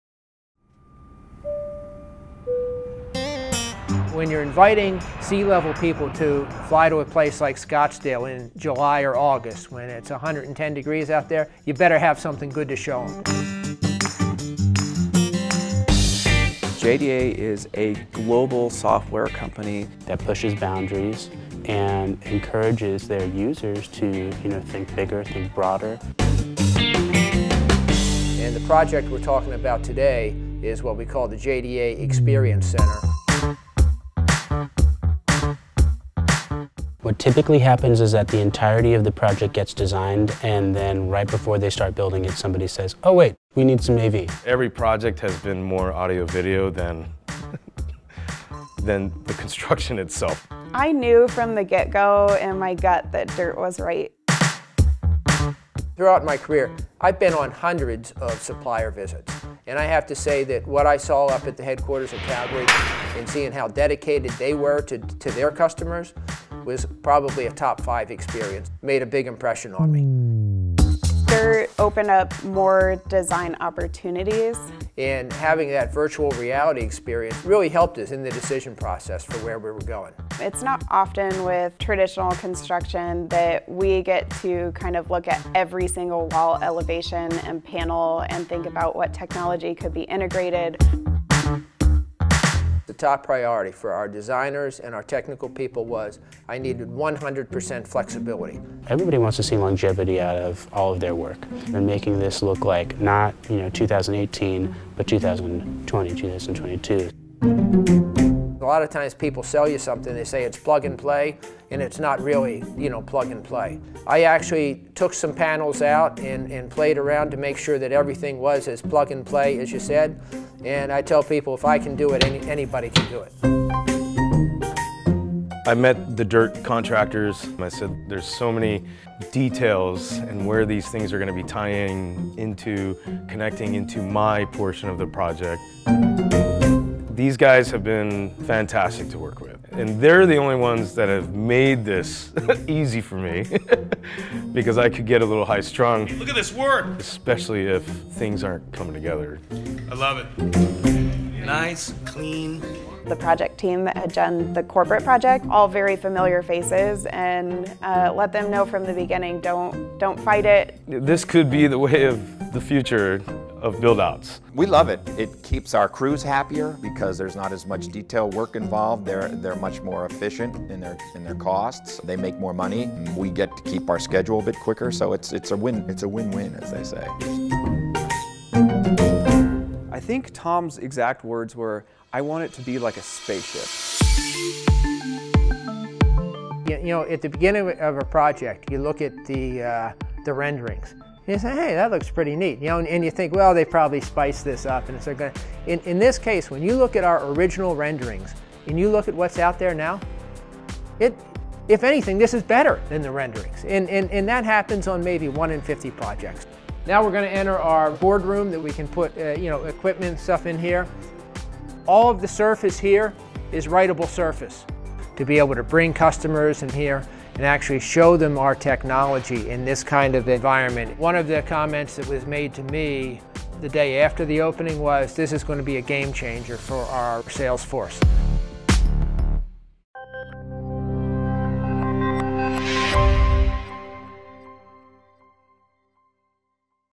JDA Software Case Study
Learn how JDA Software made a/v and adaptability the lynch pins for their new client experience center of their HG in Arizona. The look and feel already has clients further along the sales cycle faster then ever before